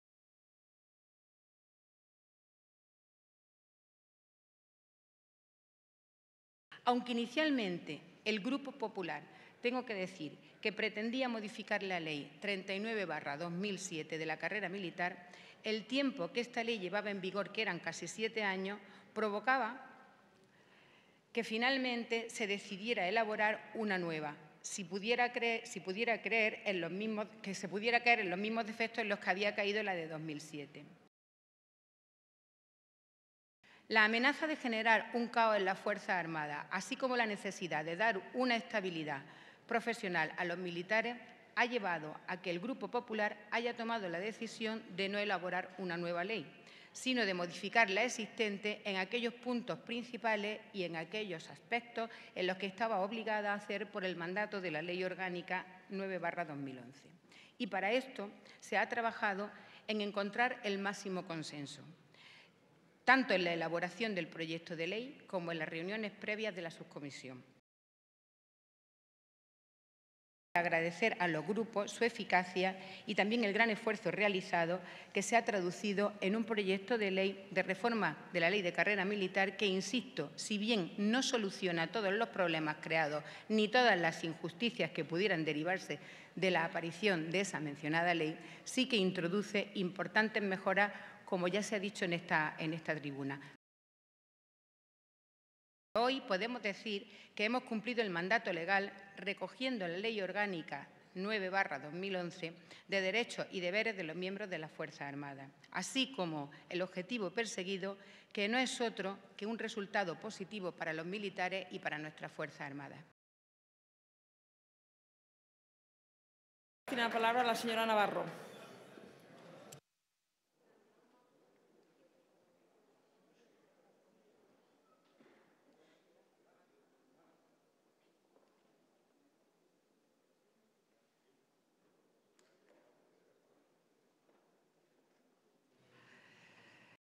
"La modificación de la Ley de la Carrera Militar supone un cambio positivo para los militares"
La diputada del Grupo Parlamentario Popular Carmen Navarro durante el debate del Proyecto de Ley de modificación de la Ley de la Carrera Militar en el Pleno del Congreso.